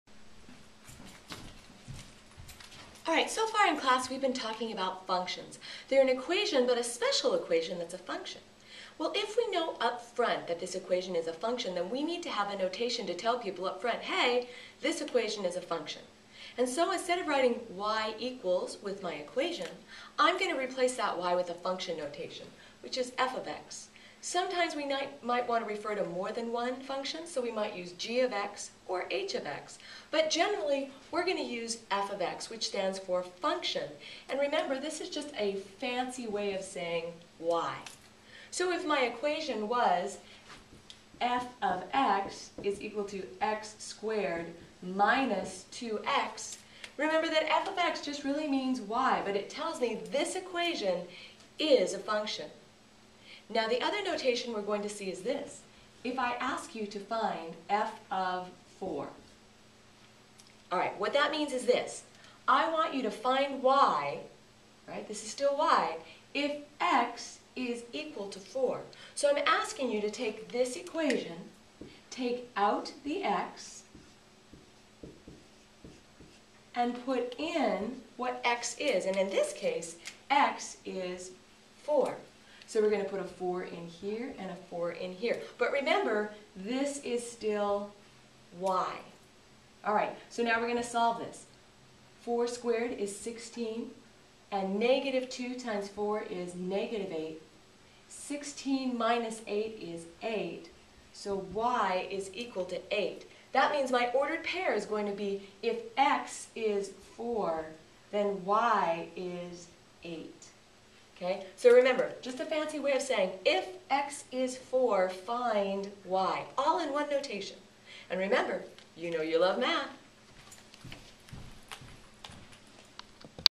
Function Notation
What does the function notation mean and what do I do with that number?